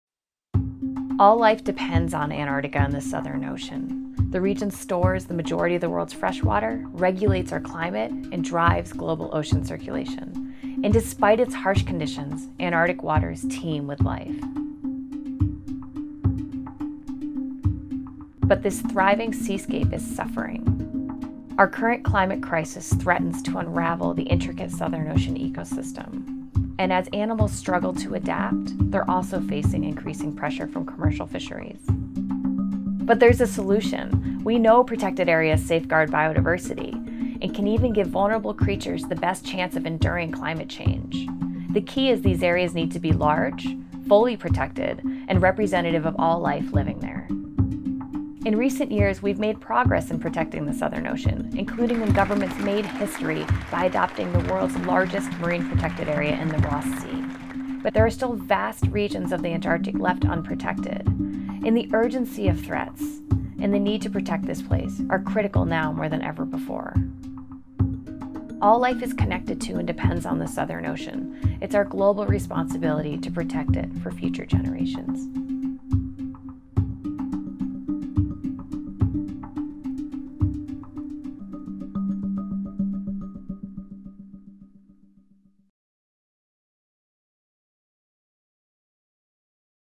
Protecting the Southern Ocean
All life depends on Antarctica and the Southern Ocean. The region teems with life, but is suffering from climate change and threat of overfishing. Protected areas can safeguard biodiversity and give life the best chance of enduring environmental change. In recent years, governments have made progress in in protecting the Southern Ocean, but as our recent research shows, there are still vast areas of the Antarctic in urgent need of protection. see our new study "Progress towards a representative network of Southern Ocean protected areas